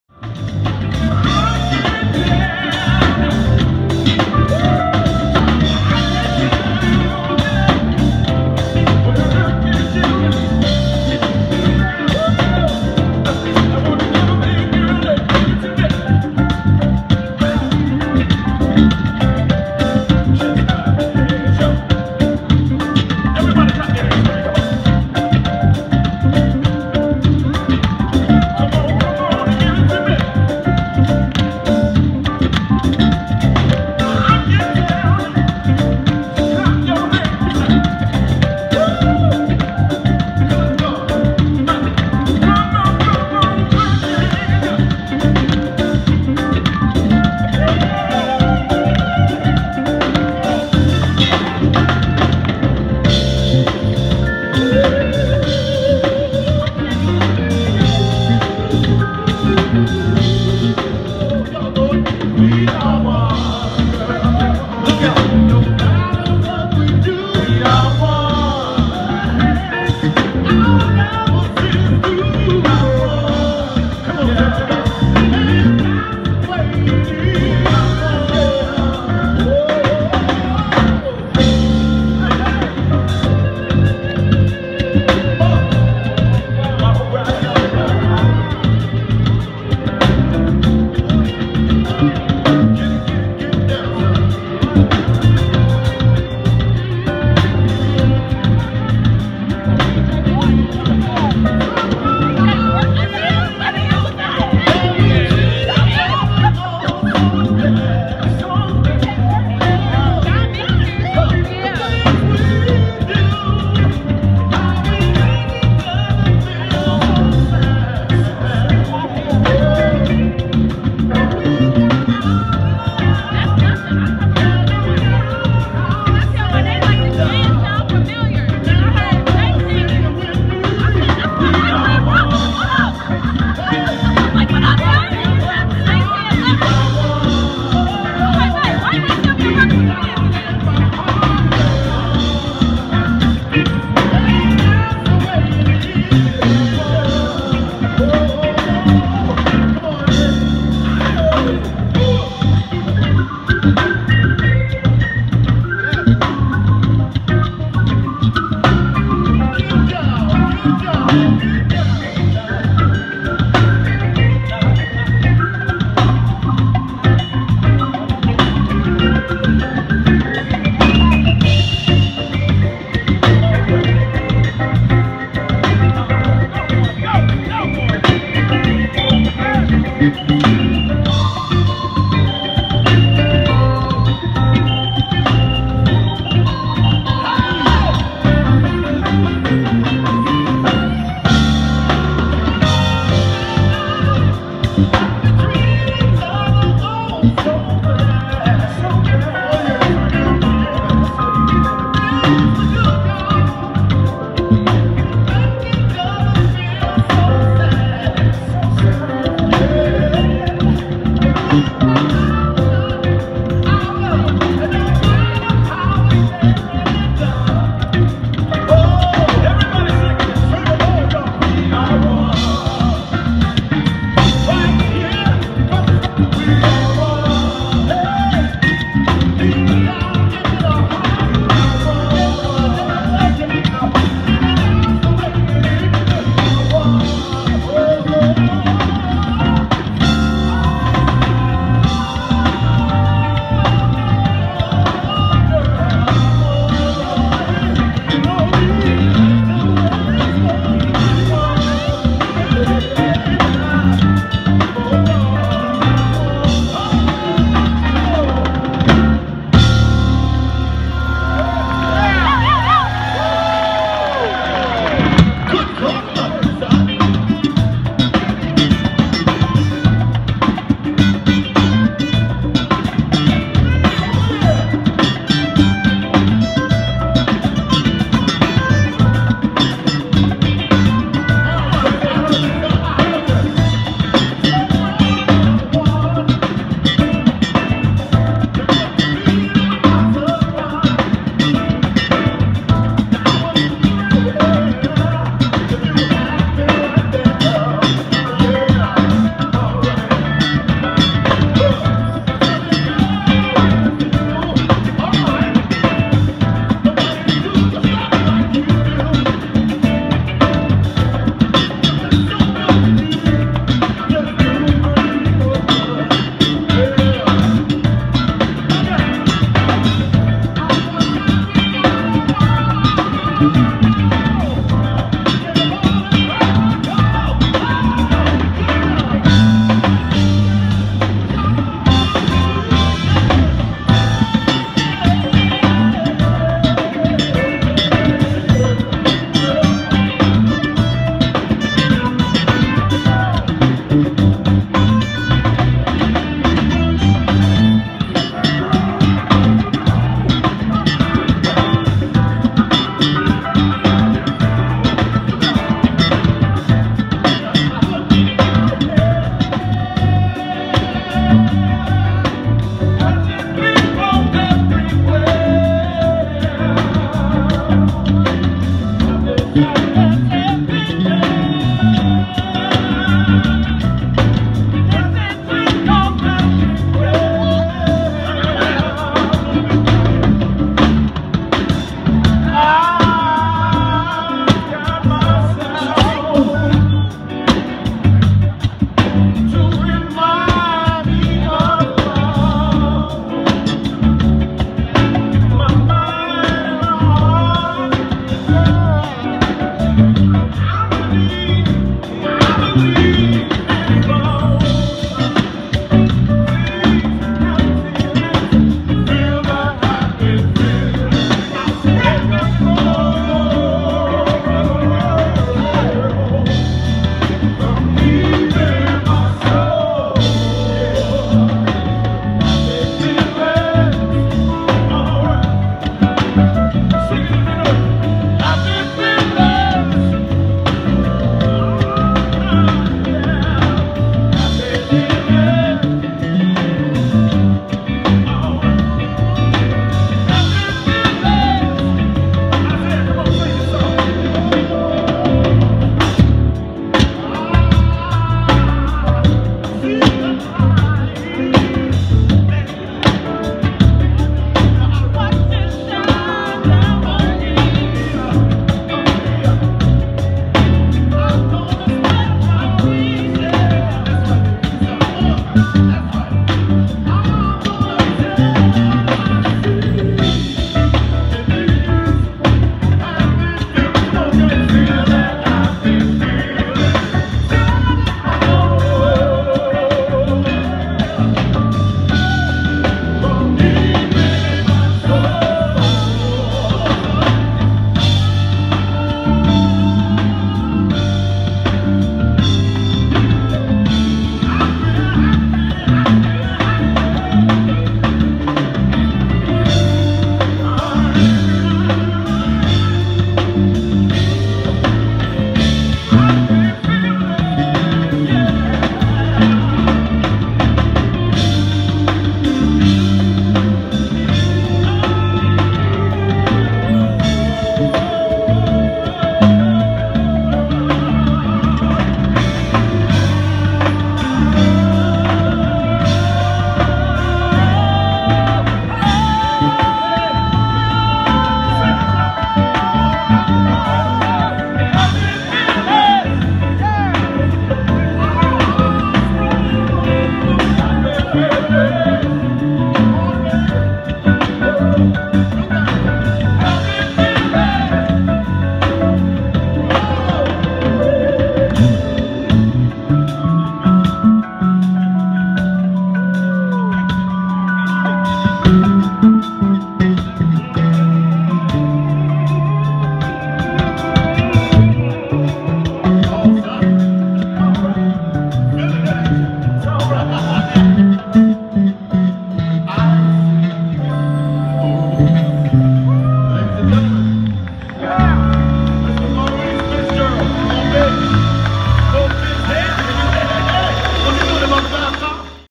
The Mo Fitz Project
February 19, 2017 The Mo Fitz Project took the stage at the Lexus Listening Lounge during the Chicago Auto-show located at The McCormick Place. Leader and Bassist Maurice Fitzgerald and the crew takes the people on a soulful journey with some of the hits from Maze Featuring Frankie Beverly.